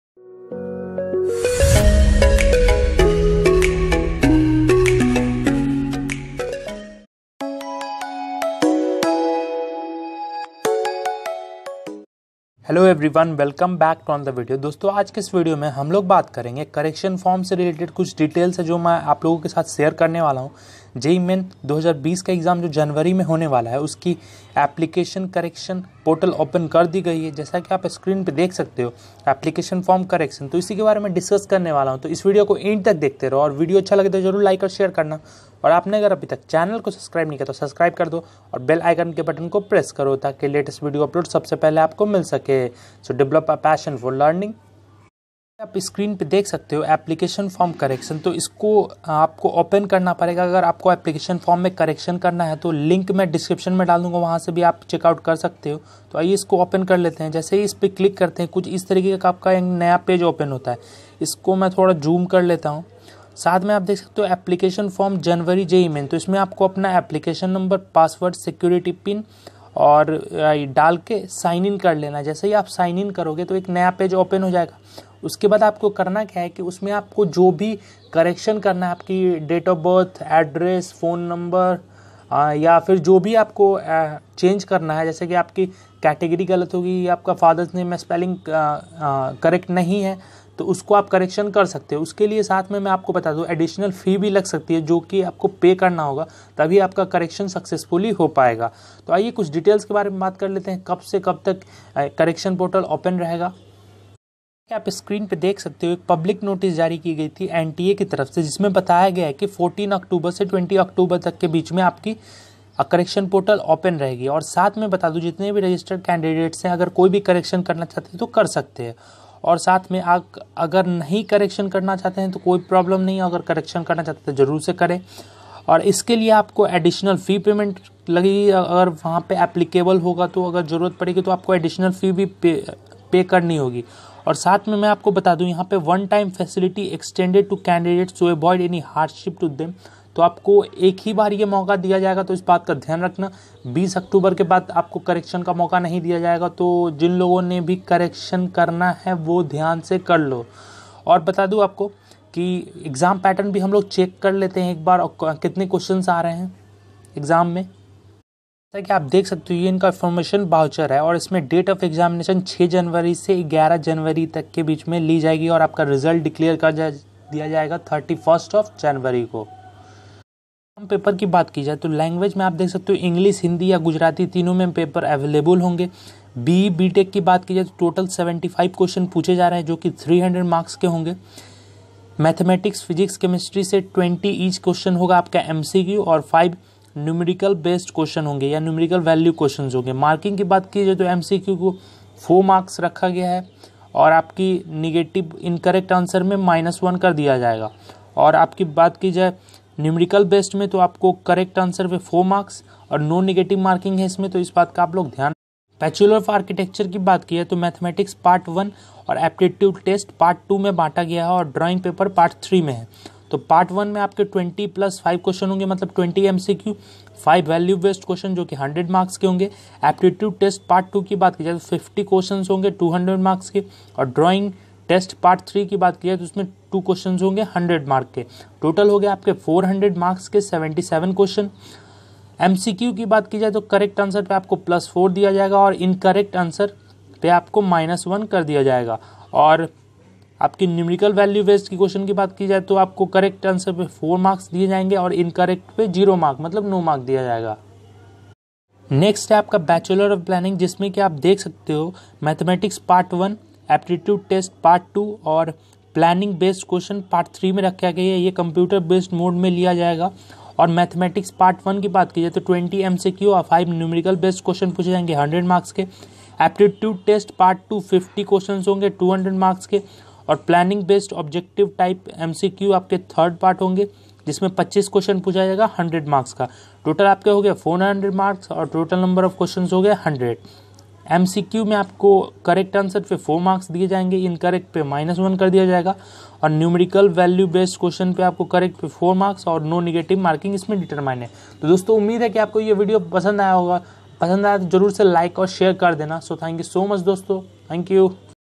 JEE Application Form Correction | Important Dates | Exam Date | Exam Pattern
PaF Shop@

Hi Everyone Dosto, Aaj ke iss video mein Maine baat ki hai JEE mains ke correction portal 14 October se 20 October Tak ke bich mein open Rahegi exam kab hai aur exam pattern ke bare mein baat ki hai. Mujhe umeed hai ki yeh video aapko pasand aayega.

More Videos to Know !!!

About: Pallavashu Films is a Youtube Channel,Where you find Education videos in Hindi, New Video is Posted Everyday -_-